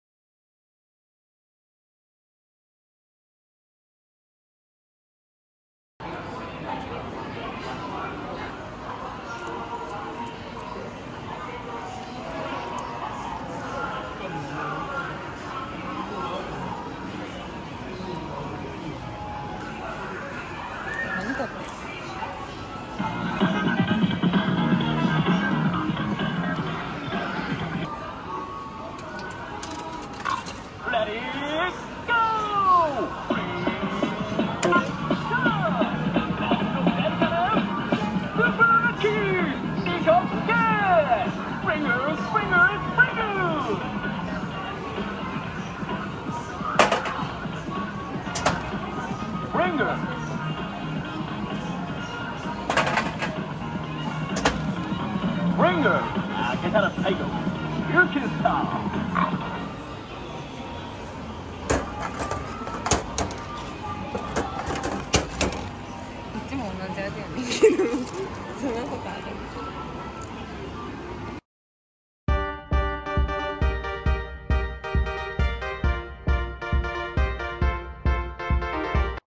PRINGLES VENDING MACHINE in Japan
Trying pringles vending machine in Japan
I love potato chips. 〜　Bonjin Diary  〜